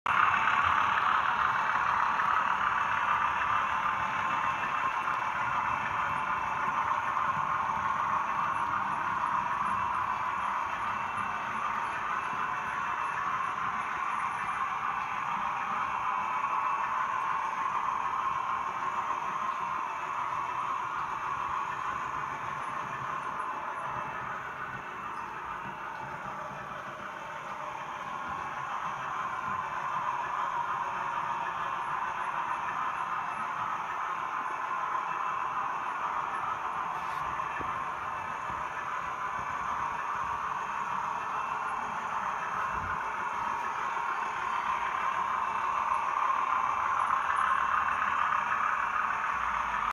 Colmore TMD
Little look round my latest depot build and the growing fleet of dutch 37s too.
Stil alot more to come with this depot build along with yard lighting towers...